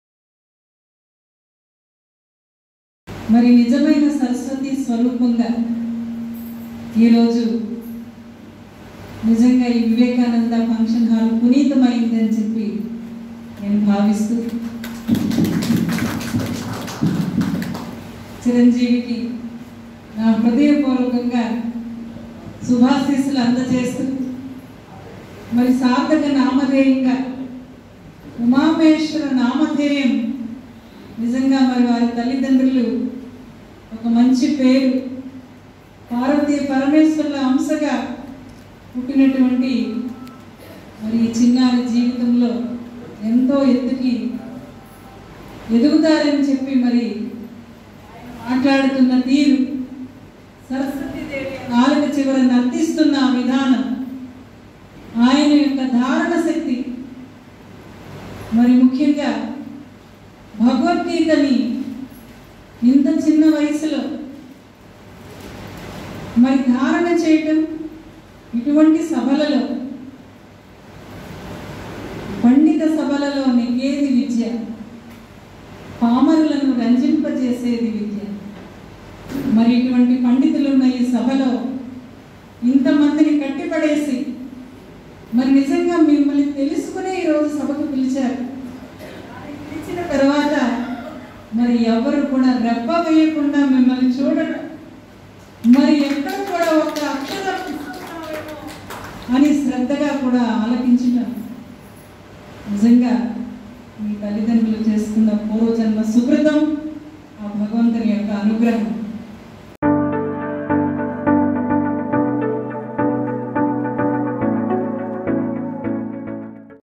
Anakapalli "M.P." Dr.Satyavathi about @UmamaheswararaoYarramsetti | Gitavadhanam | 16-4-23
Andhra Padya Kavitha Sadhaassu, Anakapalli organized the Gitavadhana on 16th April 2023. The chief guest for this program was Honourable Anakapalli M.P. Dr. K. V. Satyavathigaru and Dr. K.Vishnumurthy, Chairman of Sri Vivekananda Charitable Trust participated as Priyatithi. Both who had many responsibilities, enjoyed watching the Avadhana with great interest without moving from the stage from the start of the Avadhana to its complete end.

ఆంధ్ర పద్య కవితా సదస్సు, అనకాపల్లి వారి ఆధ్వర్యవంలో 2023 ఏప్రియల్ 16వ తేదీన గీతావధానం చేయడం జరిగింది. ఈ కార్యక్రమానికి ముఖ్య అతిథిగా అనకాపల్లి ఎం.పి. డా.కె.వి.సత్యవతిగారు, ప్రియాతిథిగా శ్రీ వివేకానంద ఛారిటబుల్ ట్రస్ట్ చైర్మన్ డా.కె.విష్ణుమూర్తి గారు పాల్గొన్నారు. ఎన్నో కార్యభారాలు ఉండే విష్ణుమూర్తి, ఎం.పి. సత్యవతి దంపతులు అవధానం మొదలైనప్పటి నుండి పూర్తిగా ముగిసేంత వరకు వేదిక నుండి కదలకుండా ఎంతో ఆసక్తితో అవధానాన్ని వీక్షించి ఆనందించారు.

ఈ కార్యక్రమానికి అధ్యక్షులుగా డా.మెరుగుమిల్లి వేంకటేశ్వర రావు, సంచాలకులుగా శ్రీ కొట్టే కోటారావు, పృచ్ఛకులుగా ఎంతోమంది నిష్ణాతులైన పండితులు, ప్రవచనకర్తలు పాల్గొన్నారు. ఈ సందర్భంగా అవధానిని, పృచ్ఛకులను, పండితులందరినీ ఘనంగా సత్కరించారు.